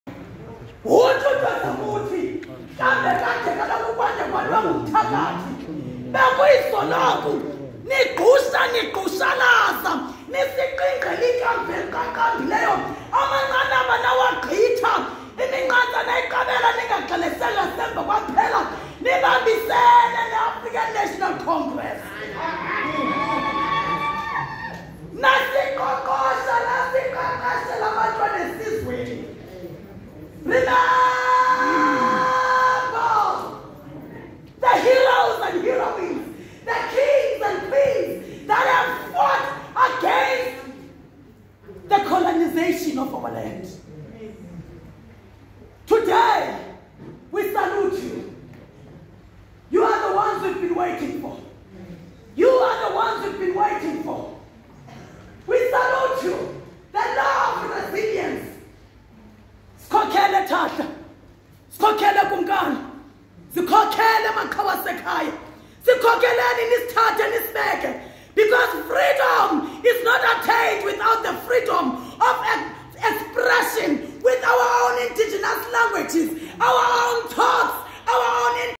Bishop Jessica Mbangeni: Honouring of Mpondo Kings and Chiefs by Freedom Park
Bishop Jessica Mbangeni rendering a poem during the event hosted by Freedom Park to honour Kung Faku, King Marhelane kaSigcau and Nkosi Ntsikayezwe Sigcau for the roles they played in resisting colonialism and in the liberation struggle.